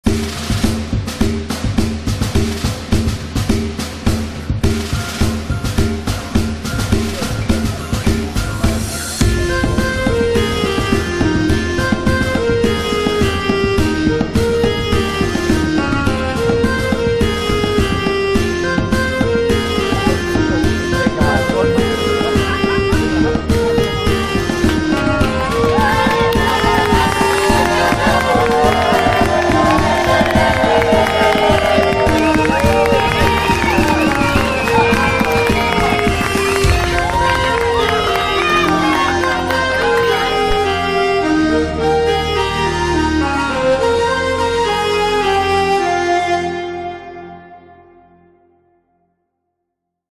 Mittelaltermarkt Heusenstamm, medival event in Germany 15.10.2023
Sehr schön, fröhlich und ausbaufähig war es!
It was very beautiful, cheerful and expandable!